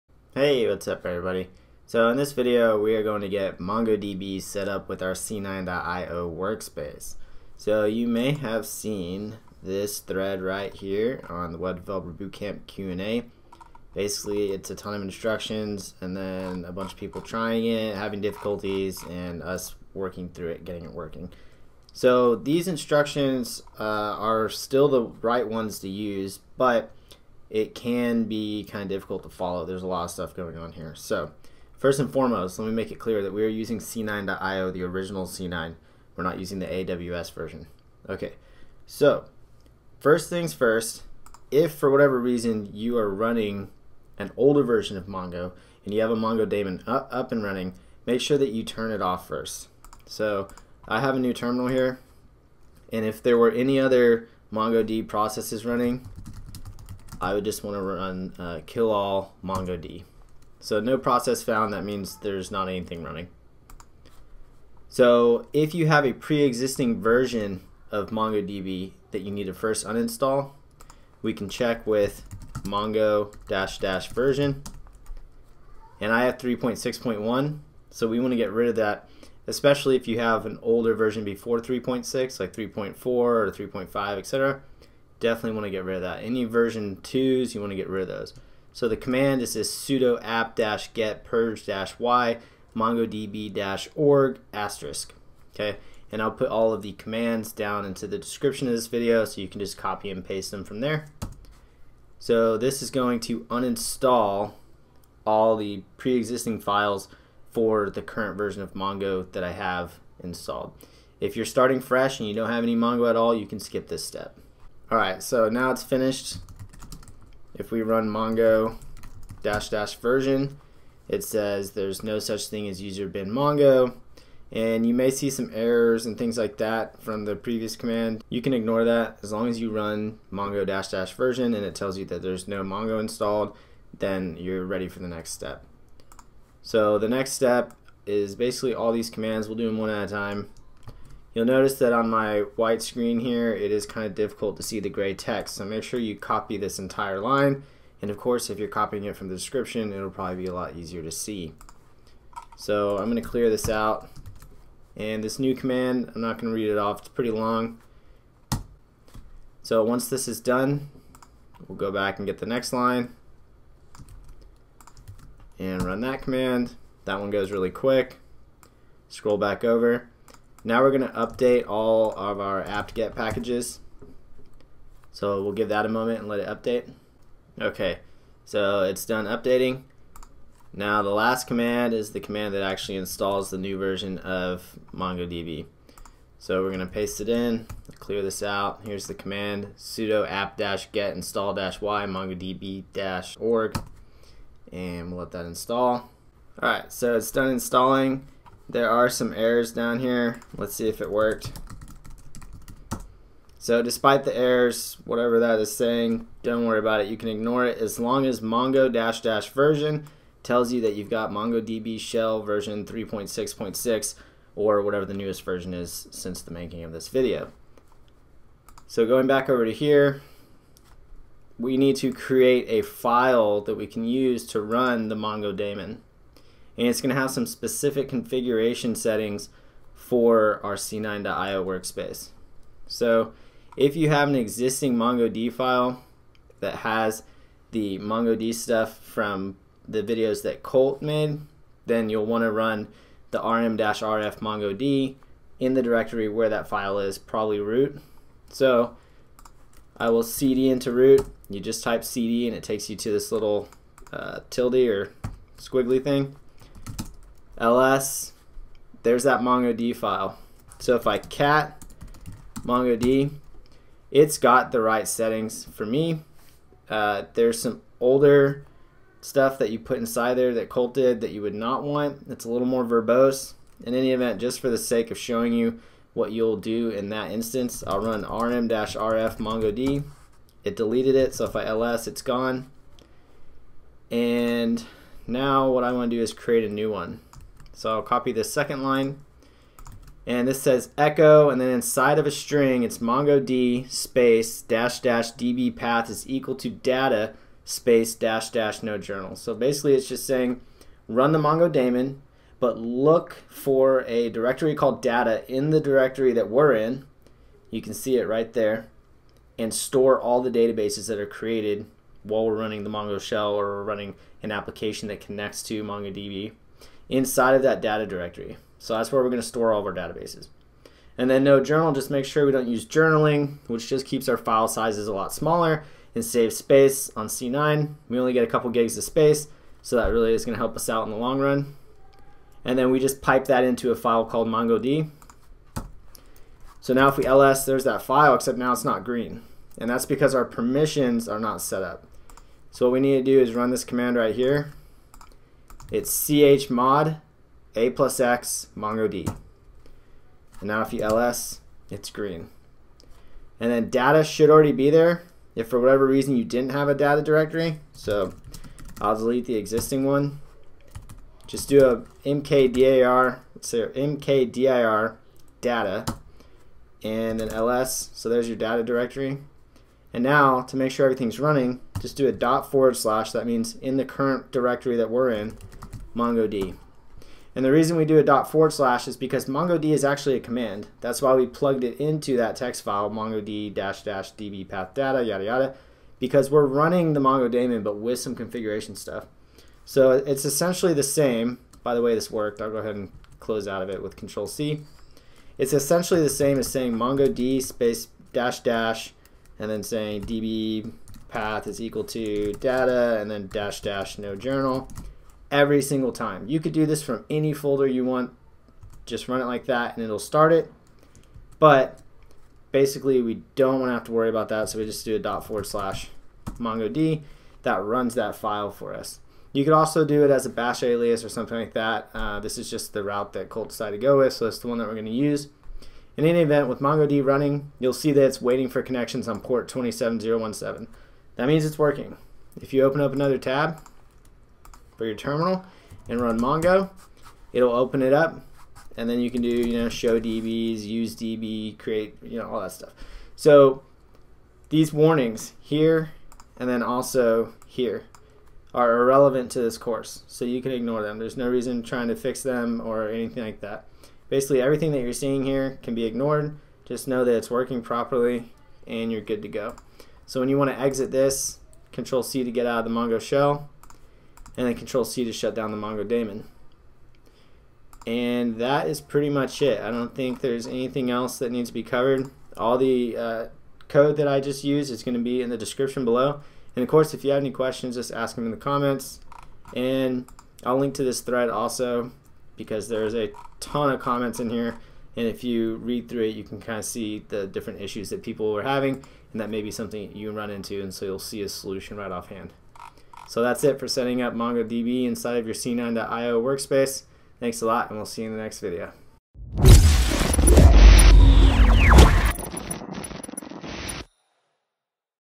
MongoDB c9.io Setup - The Web Developer Bootcamp
Learn how to setup MongoDB with c9.io (the original version) in this quick tutorial.

Thumbnail Background Photo by Ishan @seefromthesky on Unsplash